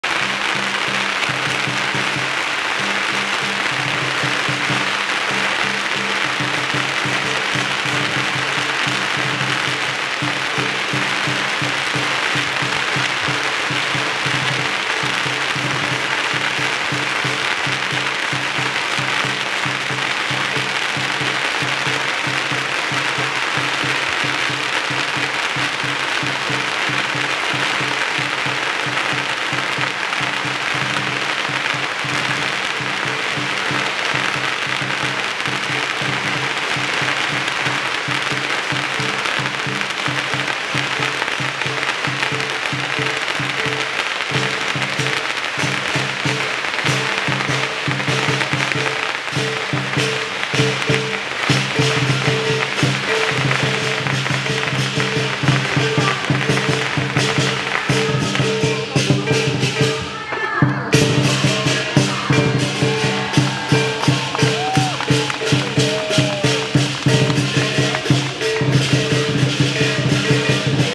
春到河畔 2024 开幕亮灯仪式 River Hongbao 2024 Opening & Fire Crackers Light Up 2/2 [4K]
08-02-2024
春到河畔 2024 开幕亮灯仪式 River Hongbao 2024 Opening & Fire Crackers Light Up
Gardens By The Bay, SuperTree Grove